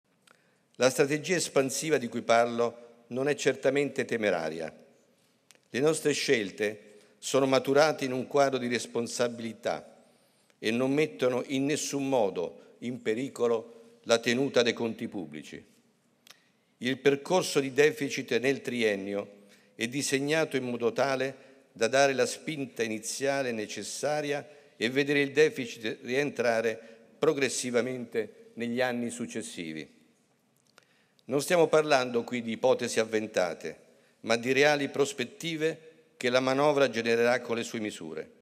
Tria: "La Manovra non mette a rischio i conti pubblici"
(Agenzia Vista) - Milano, 18 Ottobre 2018 - "Le scelte messe in campo dal Governo con il Def e con la Manovra sono maturate in un quadro di responsabilità e non mettono in nessun modo in pericolo la tenuta dei conti pubblici. Il deficit nel triennio è pensato per dare la spinta iniziale necessaria e per vederlo rientrare negli anni successivi". Lo ha detto il ministro dell'Economia Giovanni Tria.